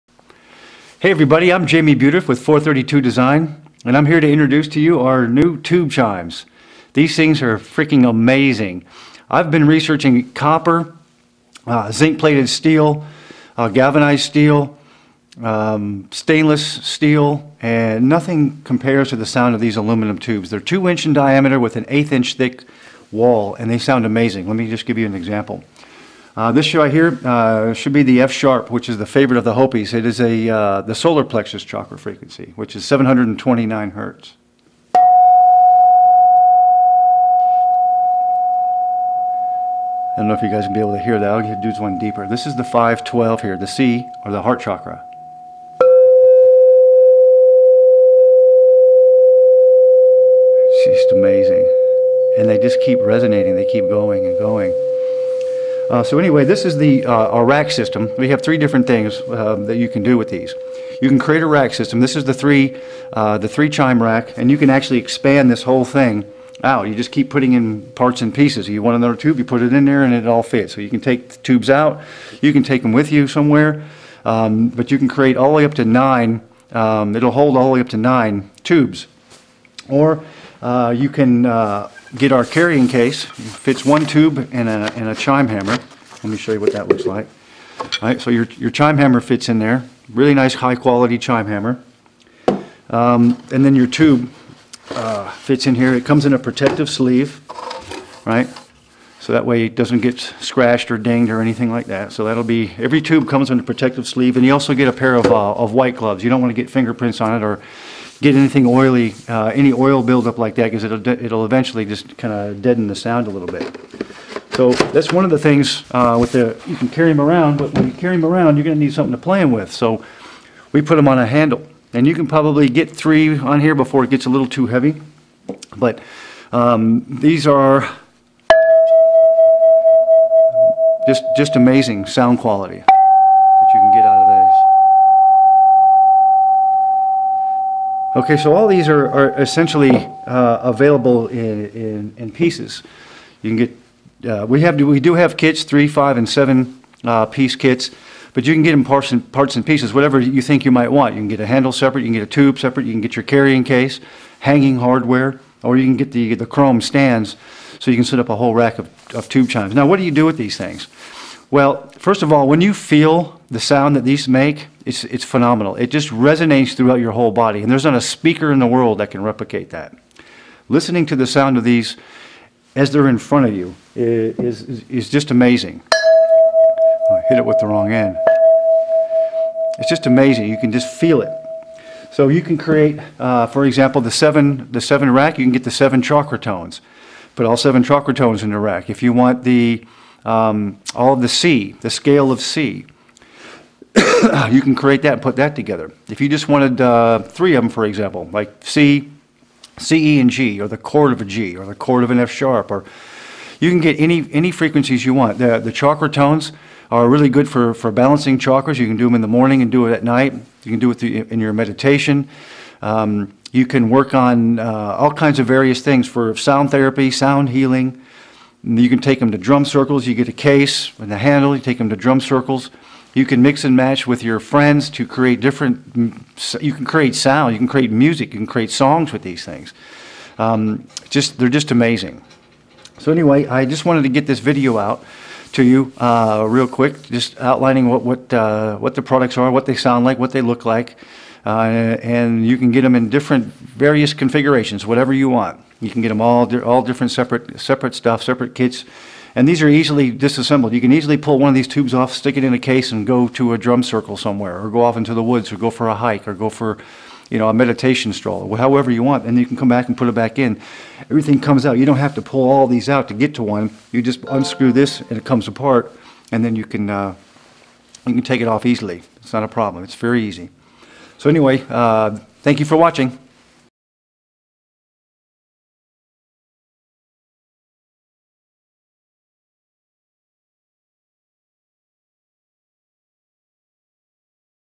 432 Design - Tube Chime Intro
Our latest creation - 2" x .125" 6061 aluminum tube chimes in the A=432Hz scale.  Precision tuned to within 0.04% accuracy in any frequency you desire.  Build a custom rack, put them on a handle, or take them with you in an adjustable carry case.